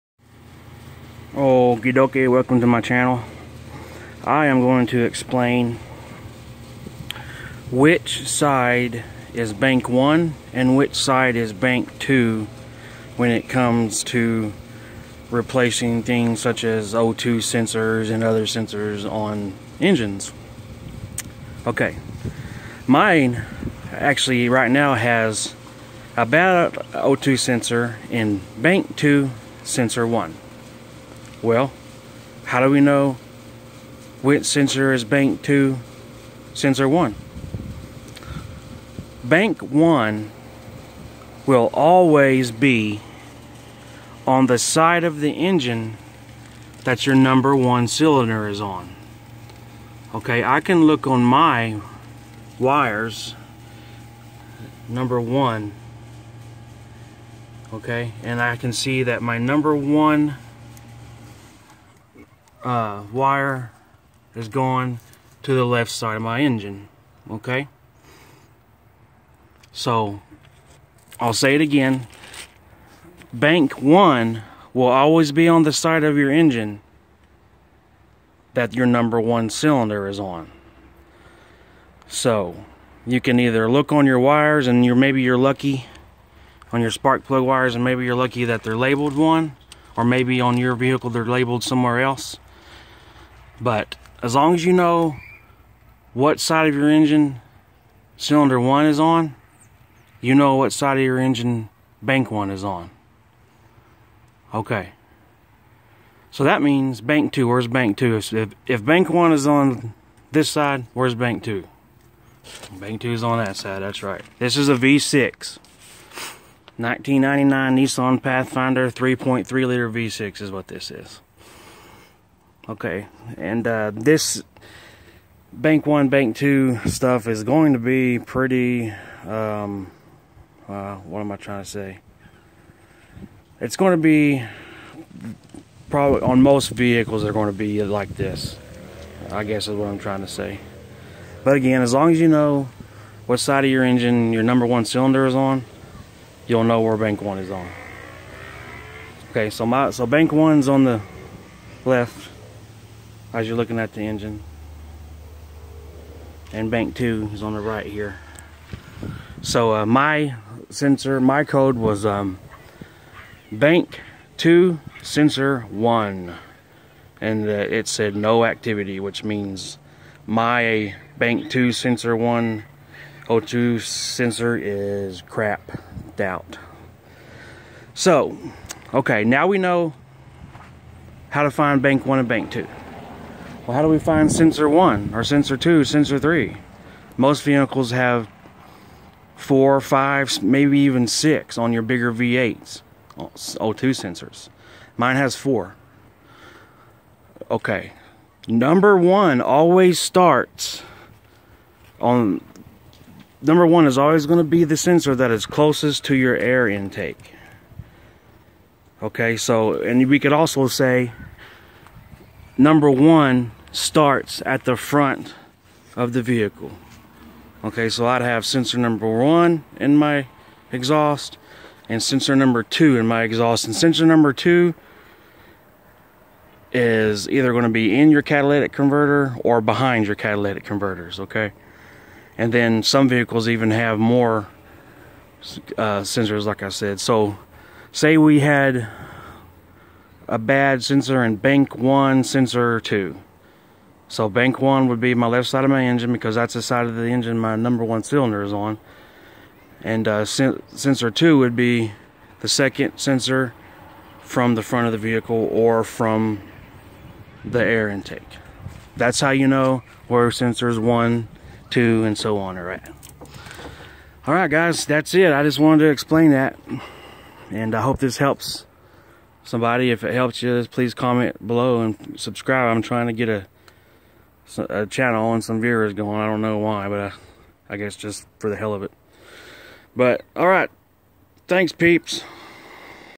How to understand Oxygen Sensor Locations (Bank 1 and Bank 2)
In this video I explain how to understand Oxygen sensor locations (Bank 1 and Bank 2).